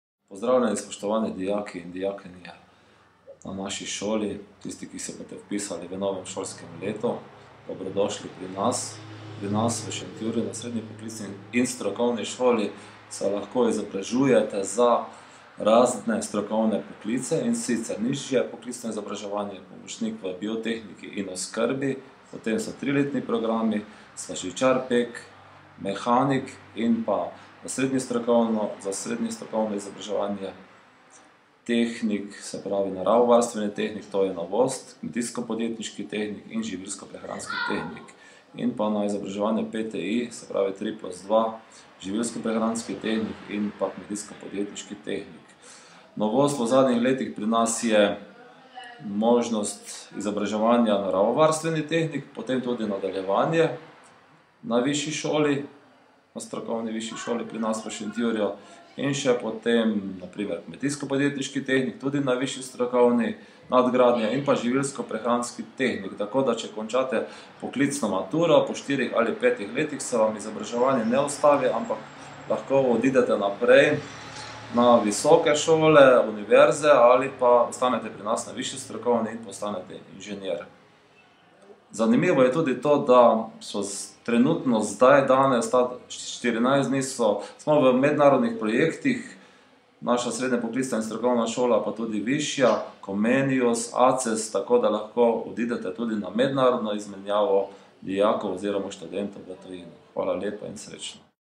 Šolski center Šentjur
Ravnatelj Srednje poklicne in strokovne šole predstavlja in pozdravlja vse, ki se želite vpisati na Šolski center Šentjur. Vse ostale informacije pa so dosegljive na: